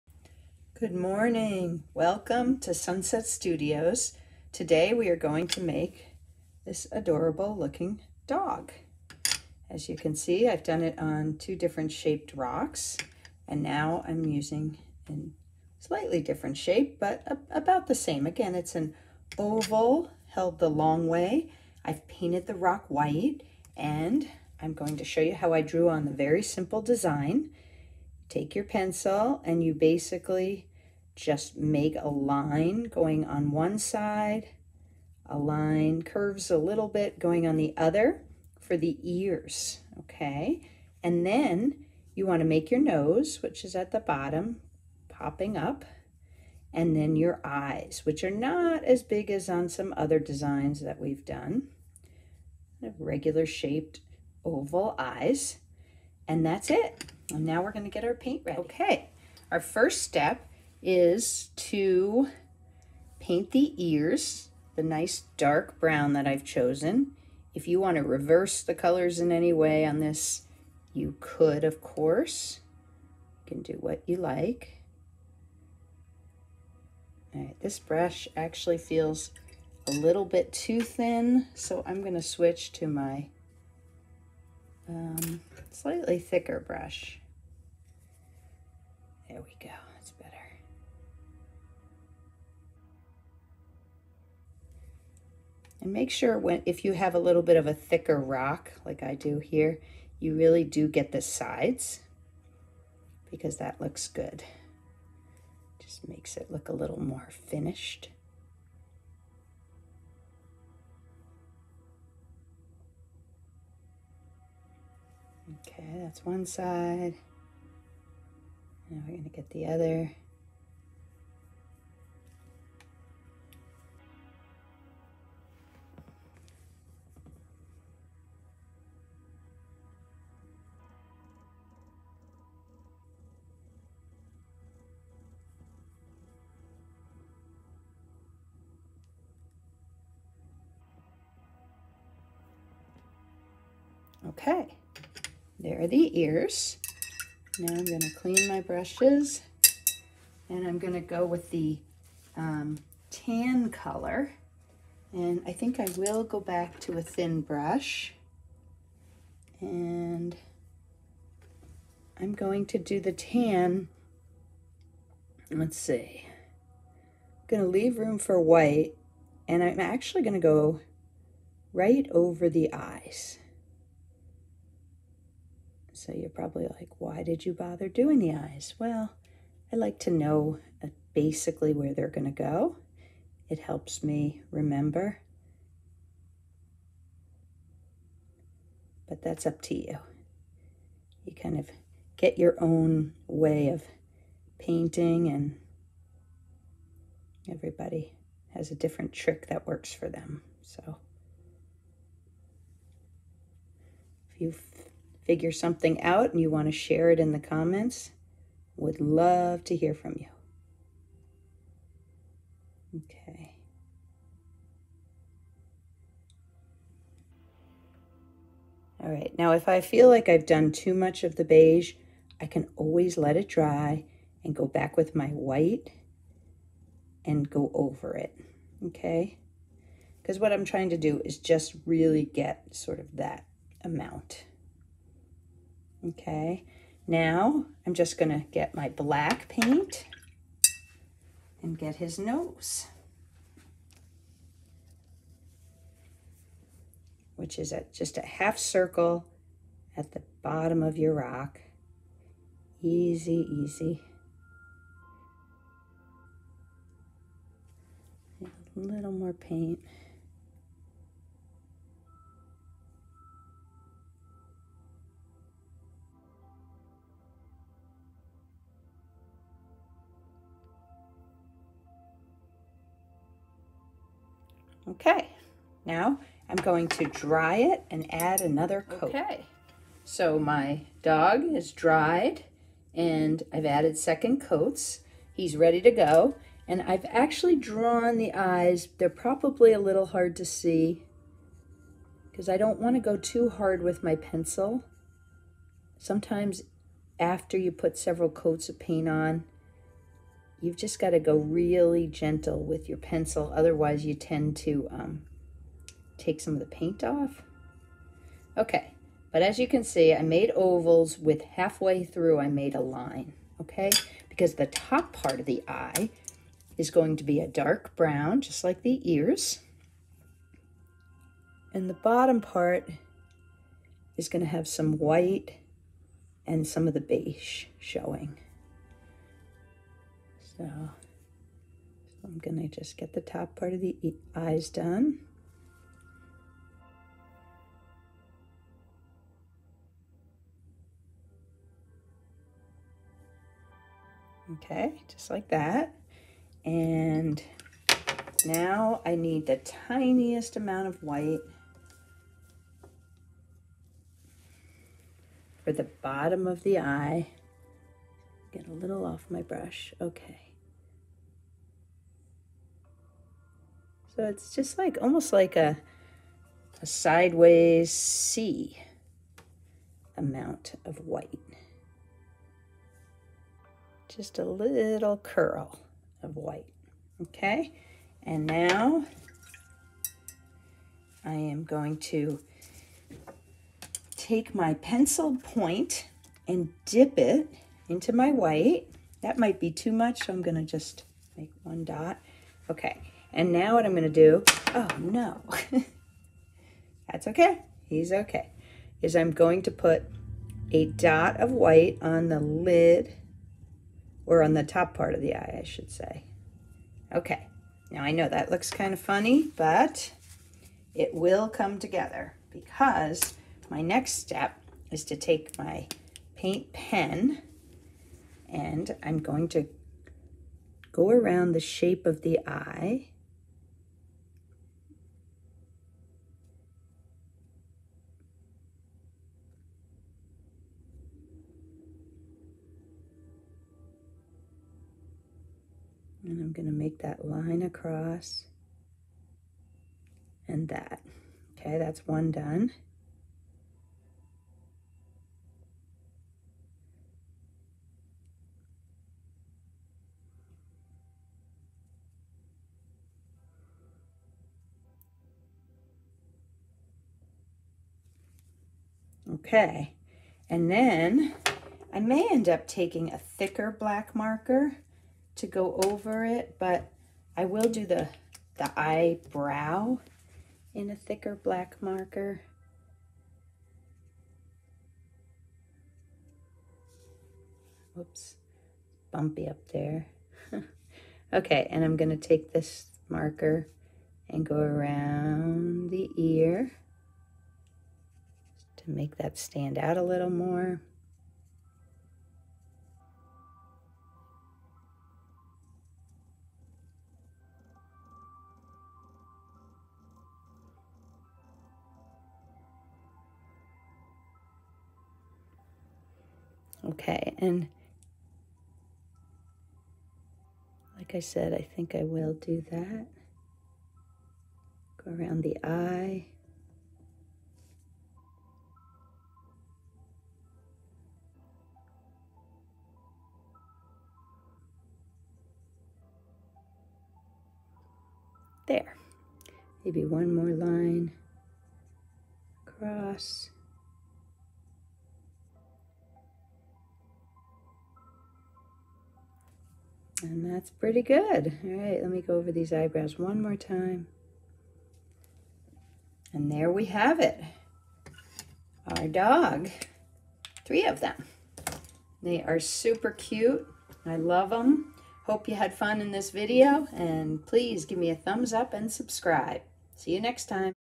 Barclay the Basset Hound (Bite Sized Art Lesson!)
Come learn this super simple way to draw this friendly basset hound.  All dog lovers will delight in this sweet face!

Materials Needed:
Smooth oval shaped stone or paper
Acrylic paint or coloring tools of your choice for paper
Paint tray
Thin and thicker paint brushes
Water
Paper towel
Black paint pen
Pencil
Eraser